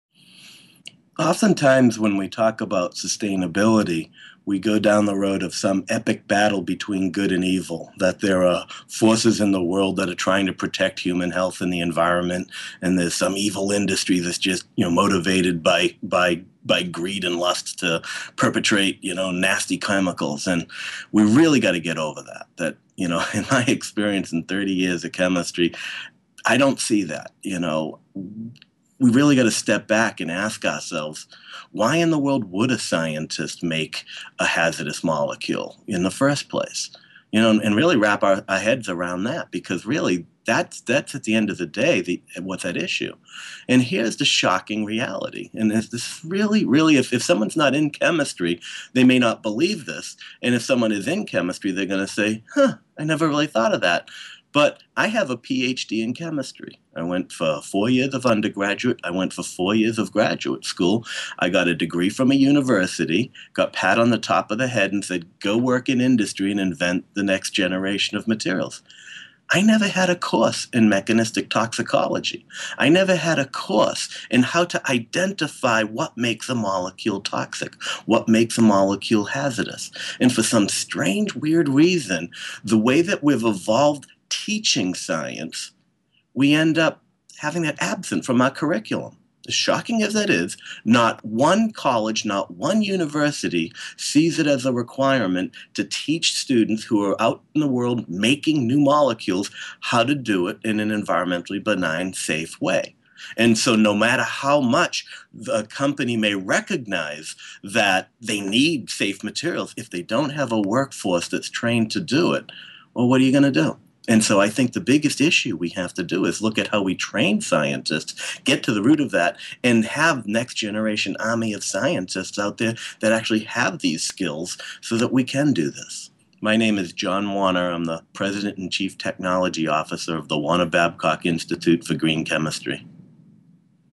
Future of Sustainable Chemistry, John C Warner
Considered one of the founding fathers of Green Chemistry, John Warner founded Warner Babcock with Jim Babcock and Bill Kunzweiler. He serves on the Board of Directors of the Green Chemistry Institute in Washington DC.